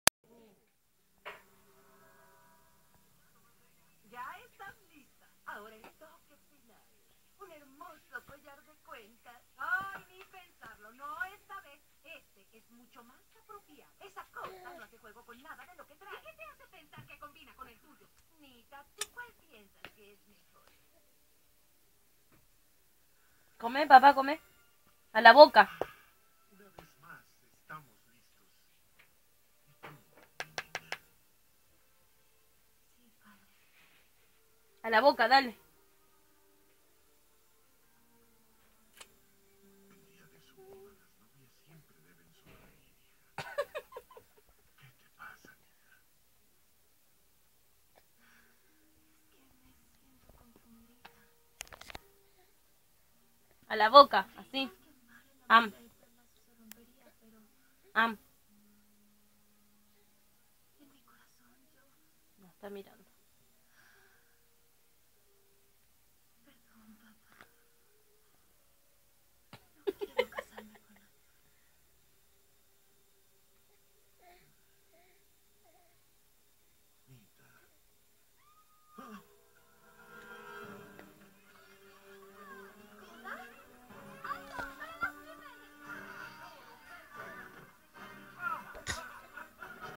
su 1ª comidita...
su primer comidita, al fin y al cabo me terminaron corrigiendo q no era pure de zapallo, si no, polenta con manteca y casancrem hecho por la mami.